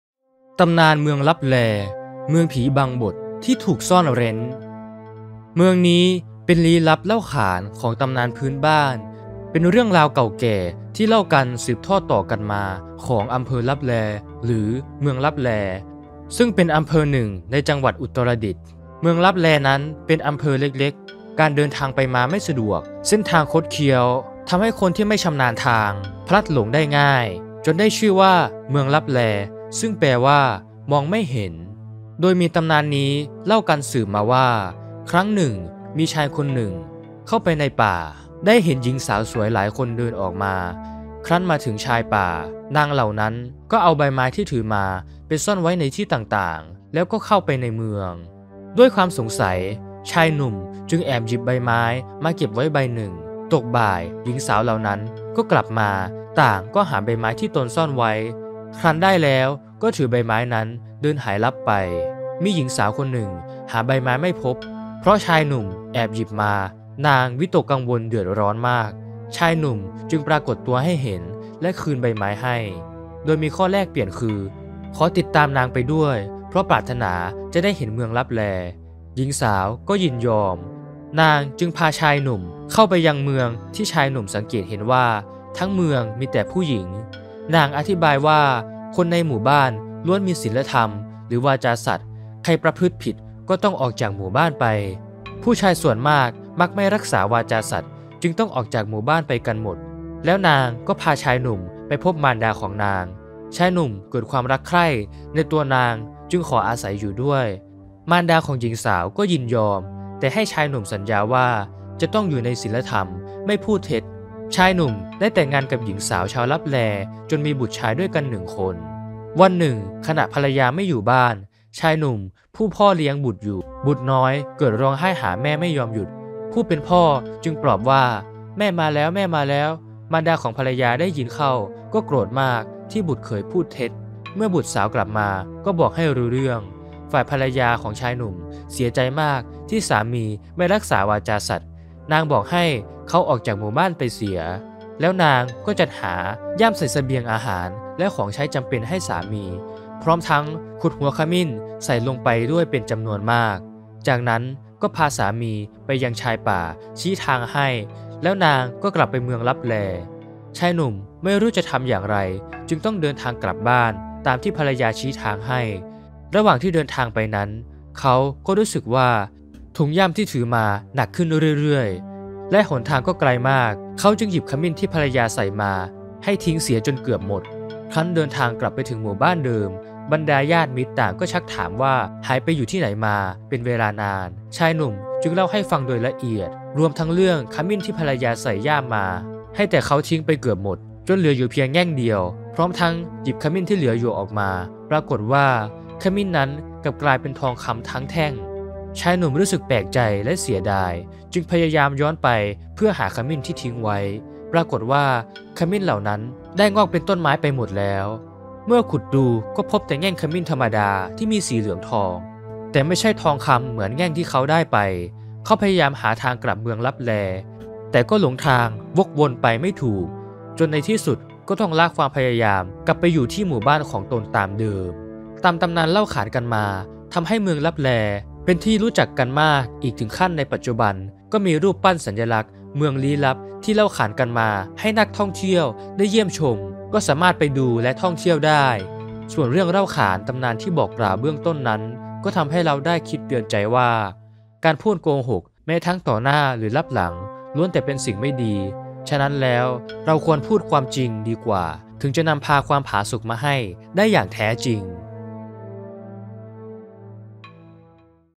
ตำนาน เมืองลับแล เมืองผีบังบด ที่ถูกซ่อนเร้น
ตามตำนานเล่าขานมาว่าเมื่อผีดิบยารามายาฮู ต้องการที่จะออกล่าเหยื่อนั้น พวกมันจะคอยแอบซุ่มแลัซ่อนตัวเองอยู่บนกิ่งไม้  ตามต้นไม้ เพื่อไม่ให้เหยื่อมองเห็น พวกมันจะเฝ้ารอคอยให้เหยื่อเดินผ่านมาต้นไม้บริเวณนั้นแล้วก็จะกระโดดลงมาเล่นงานอย่างไม่ทันตั้งตัว